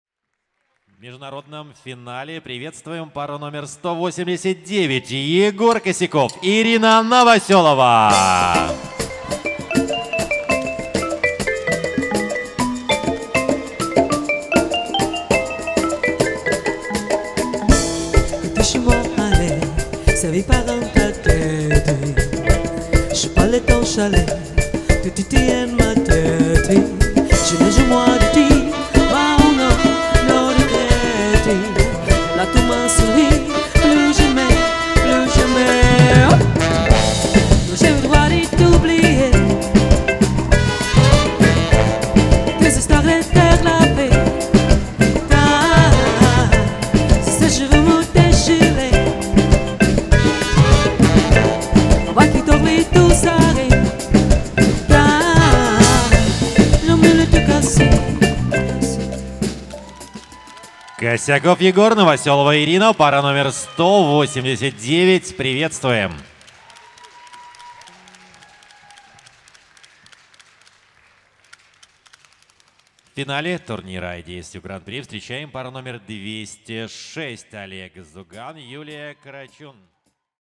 Презентация пара №189 Косяков Егор и Новоселова Ирина Minsk Open Championship – 2021
Организаторы:
3 отделение 18:00 20 февраля

пара №189 Косяков Егор и Новоселова Ирина - 6 место